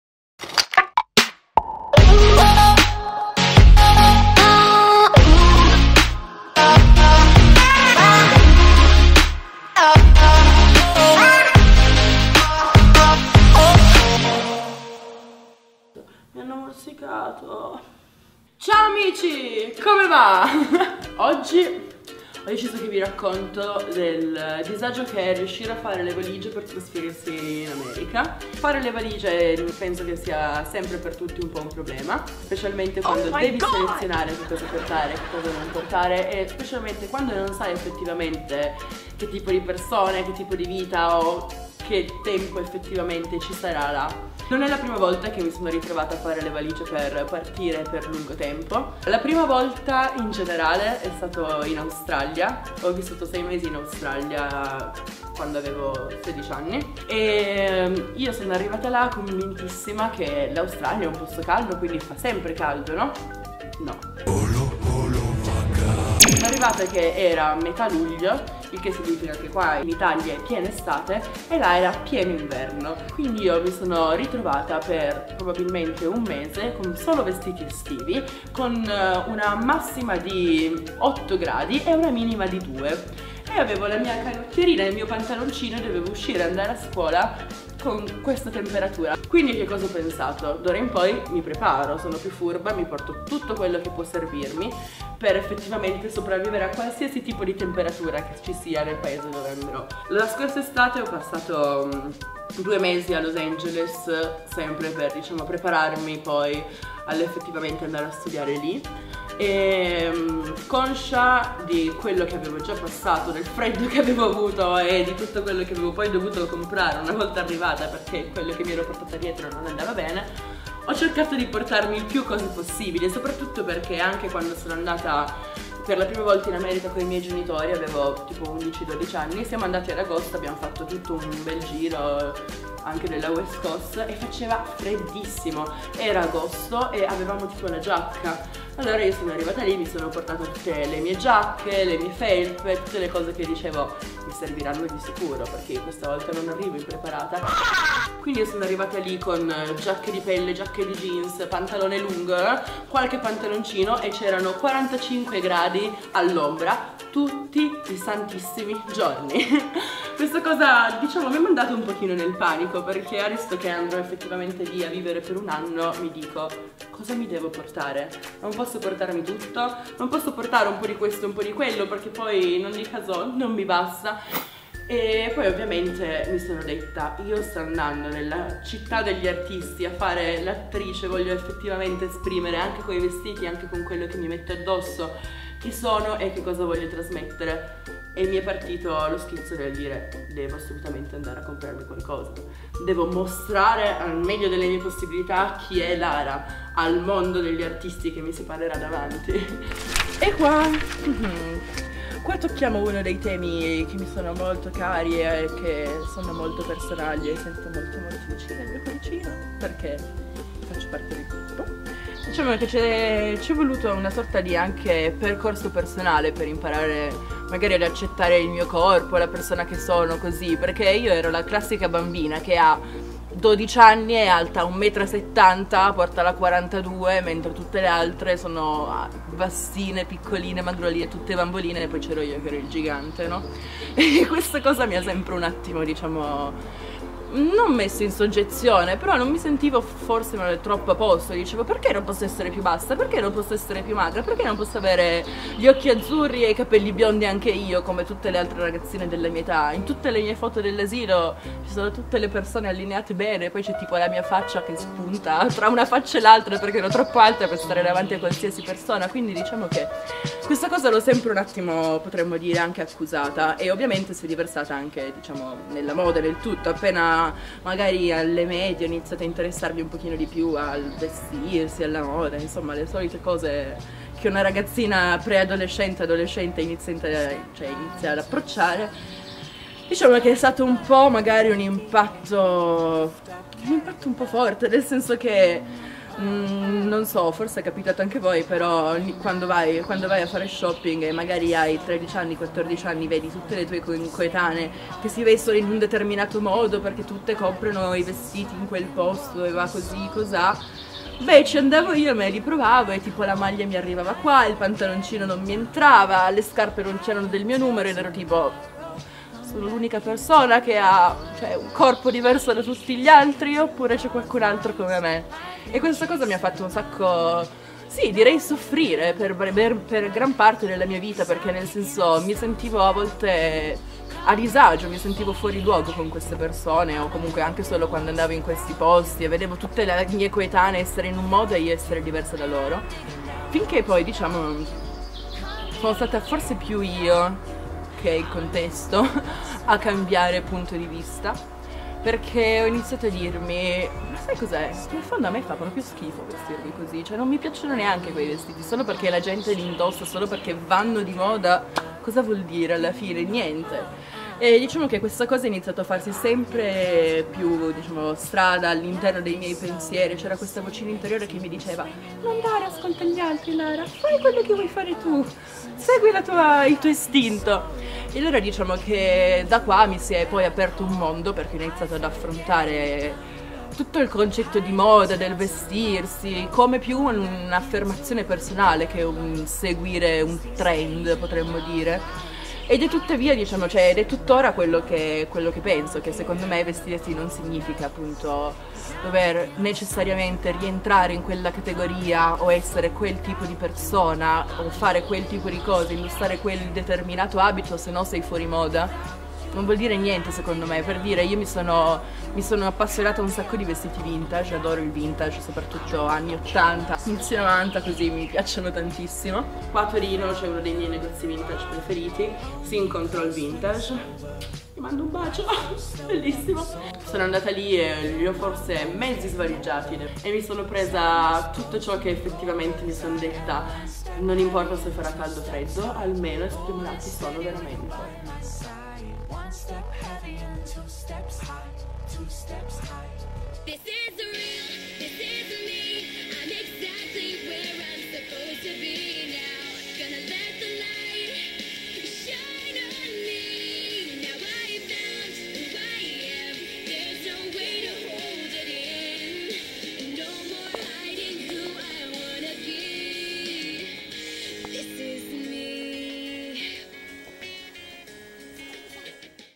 LARALAND - BAGAGLI E CONFESSIONI!
La partenza è davvero alle porte!
Ma come ci si prepara al meglio per affrontare il primo giorno della nuova vita che mi aspetta? Facciamo un riepilogo sui preparativi (tra quelli più difficili, fare una valigia che duri un intero anno 😰🙀) e tra bagagli e confessioni, si parte all’avventura!

Animazione, Grafiche ed Illustrazioni a cura di Simone icardi